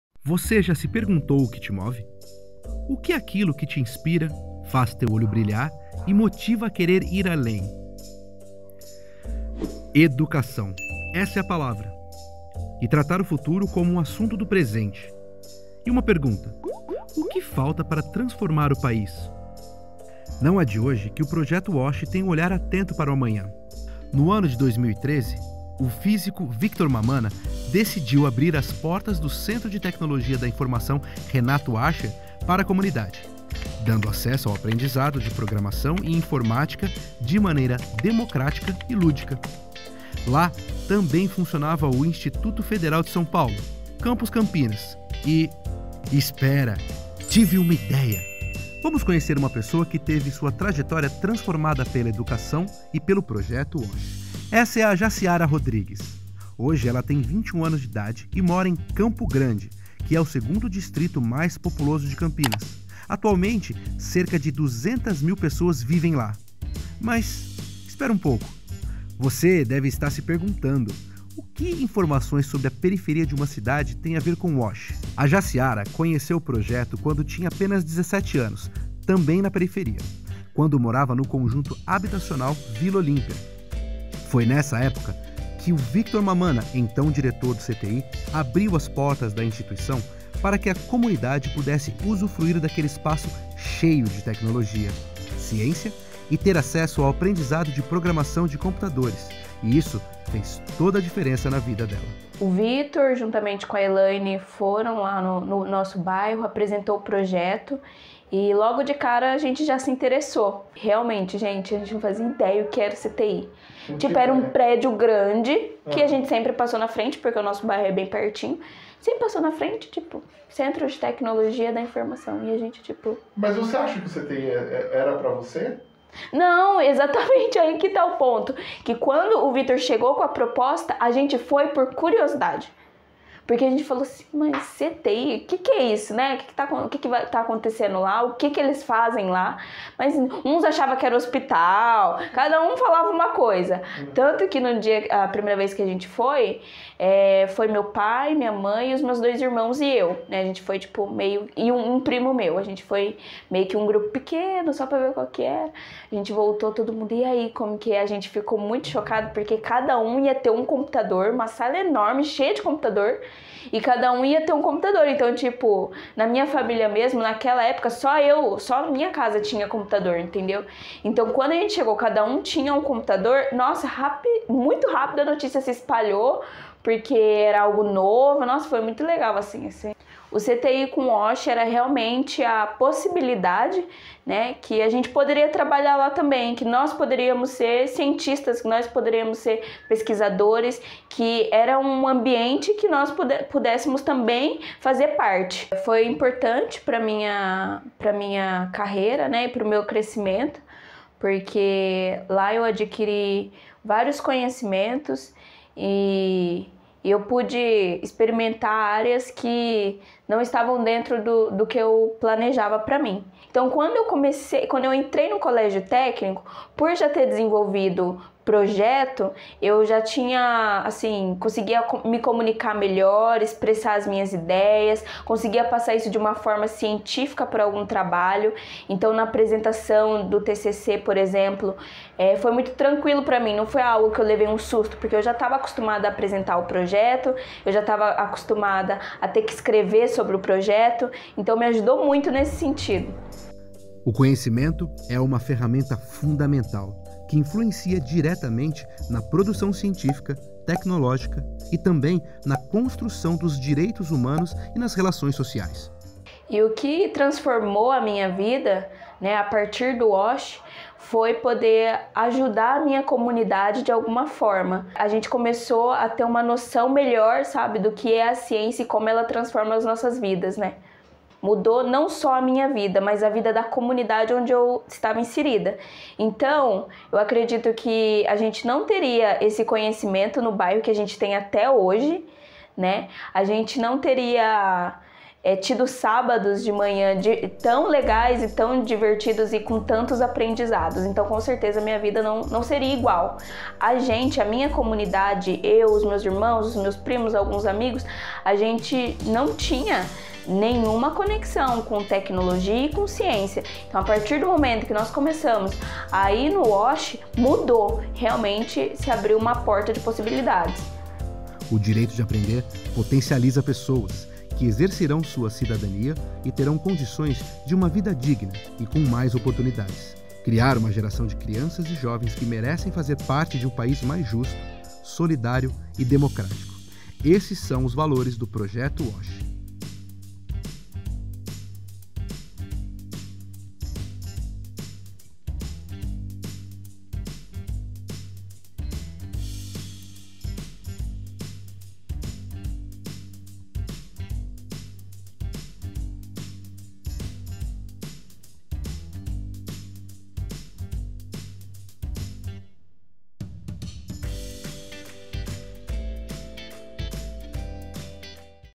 Lançamento: vídeo apresentação do WASH
Na Semana Nacional de Ciência e Tecnologia 2021, que tem como tema, este ano, a "A Transversalidade da Ciência, Tecnologia e Inovações para o Planeta", o vídeo “WASH Transforma” mostra como tudo começou.

“A transversalidade da ciência, tecnologia e inovações para o planeta” foi o tema escolhido para a 18ª Semana Nacional de Ciência e Tecnologia – SNCT, que acontece entre os dias 2 e 8 de outubro de 2021. Para marcar a data, o WASH lança seu vídeo institucional “WASH Transforma”.

WASH significa Workshop Aficionados em Software e Hardware, uma sigla que remonta ao nascimento do Projeto, em 2013, inicialmente focado na tecnologia da informação. Este foco estava de acordo com a missão do Centro de Tecnologia da Informação Renato Archer (CTI), unidade de pesquisa do MCTI (Ministério de Ciência, Tecnologia e Inovações), em Campinas (SP).

Posteriormente, com cada vez mais parceiros de diferentes áreas, o WASH passou a ter uma amplitude temática muito mais vasta, com atividades nas áreas de exatas, humanas, biológicas e de saúde.

O Programa surgiu como uma ação de voluntários. Ao abrir as portas do CTI para a comunidade do entorno, periférica, começava uma trajetória que se estende e amplia até hoje.

O Vídeo de Apresentação do WASH mostra, em uma animação e por meio do relato de uma das jovens que participaram do Projeto, como o WASH deu início às suas atividades e seu potencial transformador, por meio de atividades não formais de educação voltadas à iniciação científica e à alfabetização científica e tecnológica.

Visite wash.net.br

Siga @programawash no Instagram